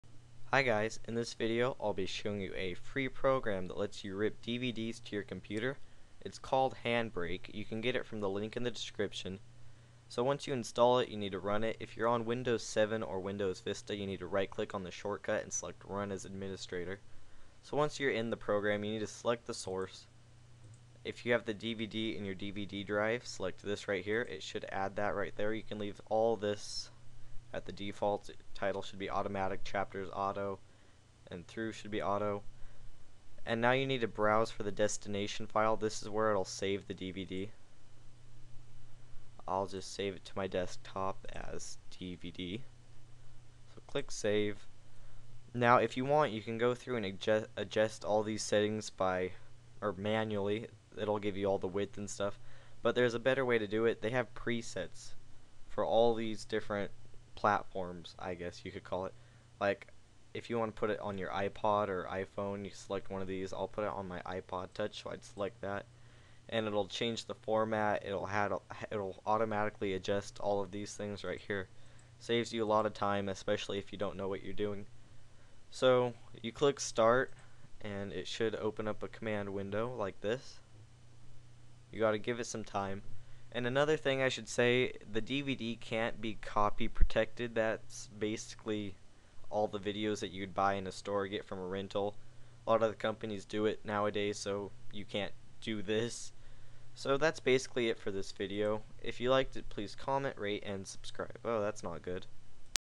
Rip DVDs to Your Computer - Handbrake
This video shows you how you can Rip Dvds to your computer. Keep in mind that this won't work for copy protected disks.